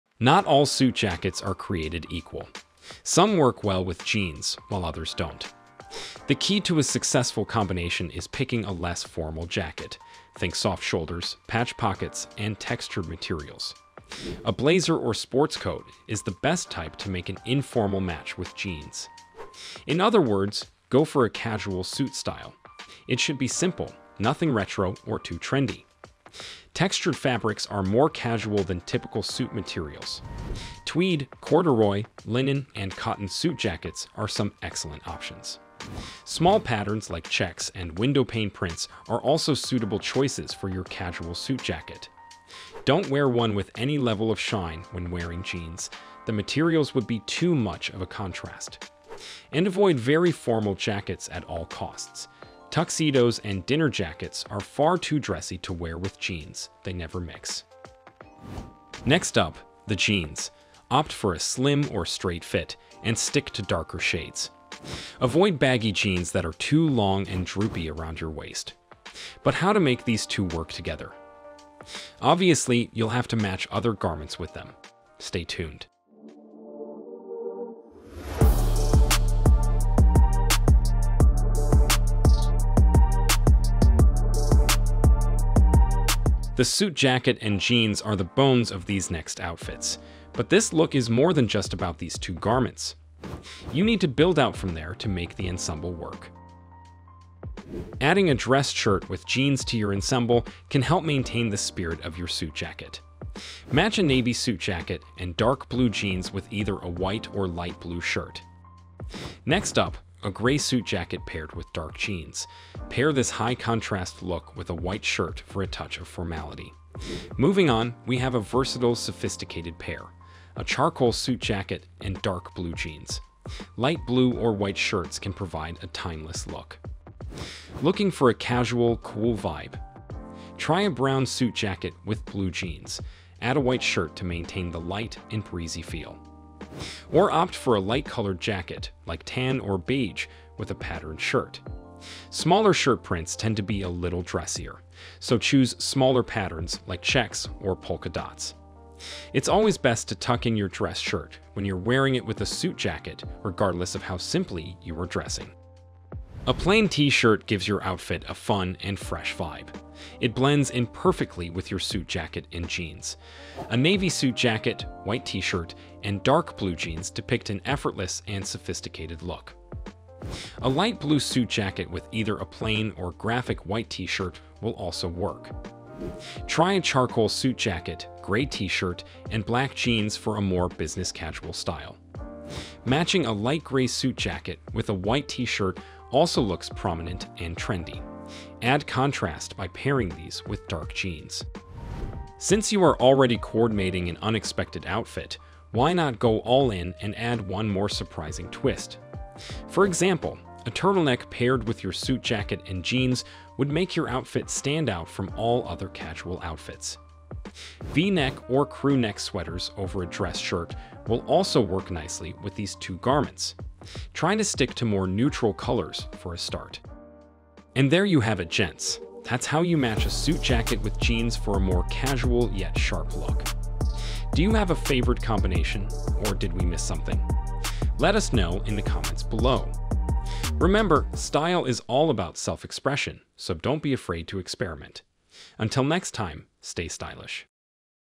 How to Wear a Suit Jacket with Jeans [Outfits for Men]
Good news - you can wear a suit jacket with jeans!

But you'll have to focus on a few things to make this look work:
1. The right suit jacket style. Choose a less formal suit jacket, like a blazer or sports coat.
2. The right fabric: Consider textured fabrics like tweed, corduroy, linen, and cotton. Small patterns like checks and windowpane prints are also suitable for a casual suit jacket.
2. Ensure a Good Fit: Both your suit jacket and jeans should fit you well.
3. Coordinate Colors: More formal suit jackets often work best with dark jeans. You can mix light blue jeans with lighter-colored suit jackets.
4. Balance the Outfit with the Right Dress Shirt or Sweater.

Want to learn more?